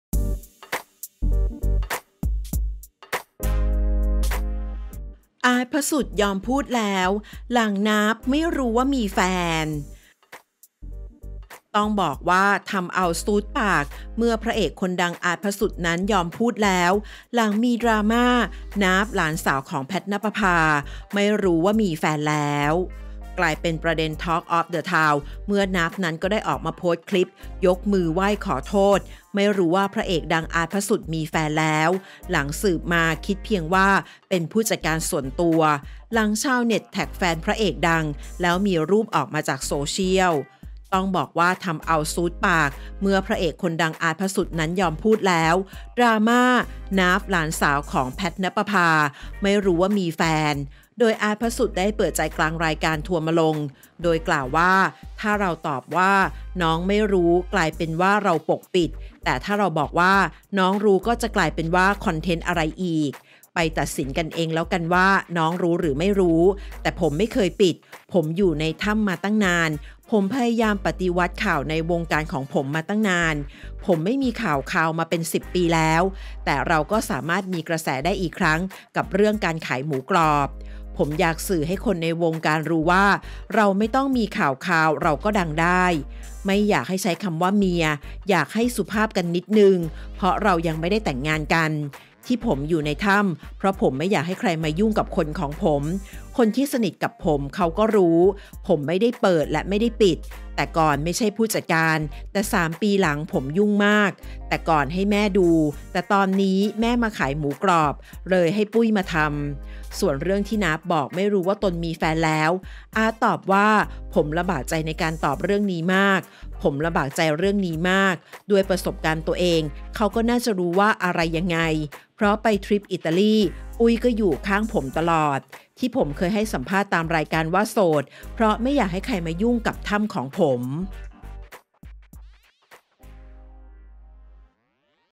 อาร์ต พศุตม์ ยอมพูดแล้ว หลัง นาฟ ไม่รู้ว่ามีแฟน
ต้องบอกว่าทำเอาซู๊ดปาก เมื่อพระเอกดัง “อาร์ต พศุตม์” นั้นยอมพูดแล้ว หลังมีดราม่า “นาฟ” หลาน “แพท ณปภา” ไม่รู้ว่ามีแฟน

กลายเป็นประเด็นทอล์กออฟเดอะทาวน์ เมื่อ “นาฟ” นั้นก็ได้ออกมาโพสต์คลิป ยกมือไหว้ขอโทษ ไม่รู้ว่า พระเอกดัง “อาร์ต พศุตม์” มีแฟนแล้ว หลังสืบมาคิดเพียงว่าเป็นผู้จัดการส่วนตัว หลังชาวเน็ตแท็กแฟนพระเอกดัง แล้วมีรูปออกมาจากโซเชียล